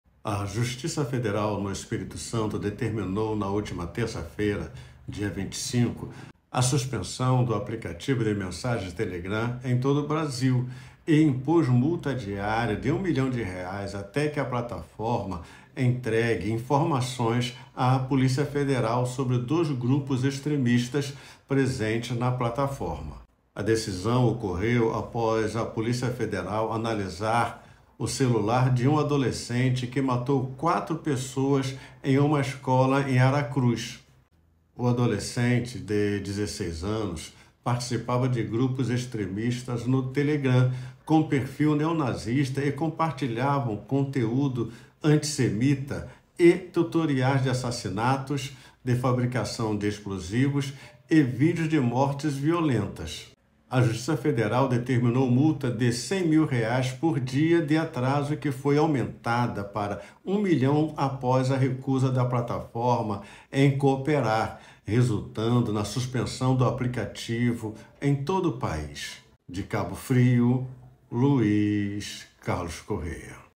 TELEGRAM FORA DO AR: O QUE MOTIVOU A DECISÃO DA JUSTIÇA FEDERAL?
O vídeo traz informações sobre a decisão da Justiça Federal no Espírito Santo de suspender o aplicativo de mensagens Telegram em todo o Brasil e impor uma multa diária de um milhão de reais até que a plataforma entregue informações à Polícia Federal sobre dois grupos extremistas presentes na plataforma. Essa decisão foi tomada após a Polícia Federal analisar o celular de um adolescente que matou quatro pessoas em uma escola em Aracruz, Espírito Santo, e encontrar na plataforma do Telegram dois grupos extremistas com perfil neonazista. Os integrantes desses grupos compartilhavam tutoriais de assassinatos, fabricação de explosivos e vídeos de mortes violentas. As investigações sobre os ataques em duas escolas em Aracruz apontaram que o autor, um jovem de 16 anos, participava de grupos de conteúdo antissemita, deixando quatro mortos e 13 feridos no final do ano passado.